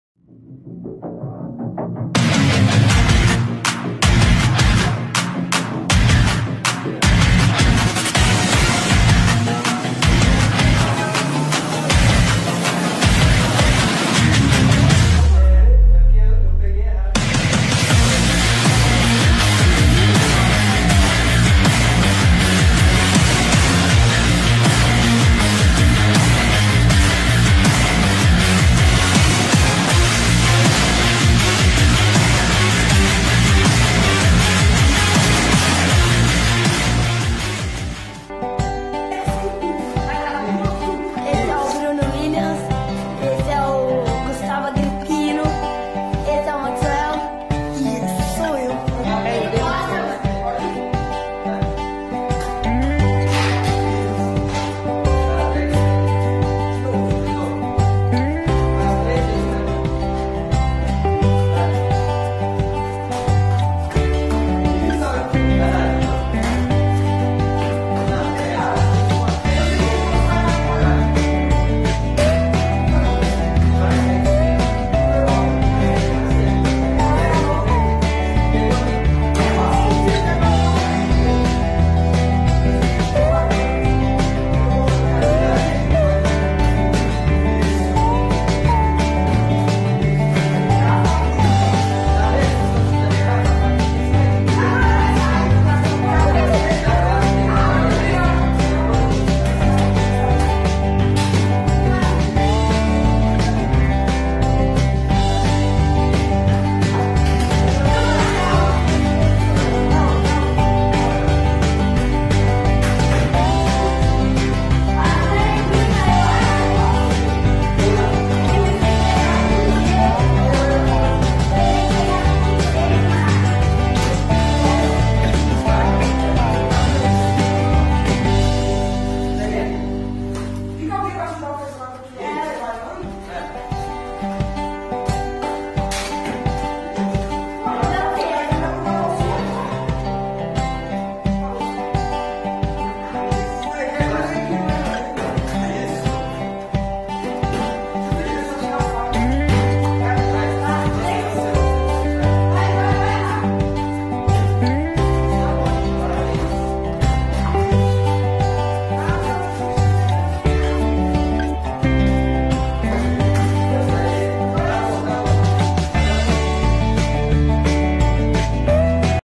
Bruno Willians teaching martial arts in En-Hacoré social project
Um homem é um sucesso se pula da cama de manhã e vai dormir à noite, e nesse meio tempo faz o que gosta.

👊😃✌☺☺☺☺☺☺ Projeto Social En-Hacoré Onde sou voluntário dando aulas de dança hip hop e TaeKwon-Do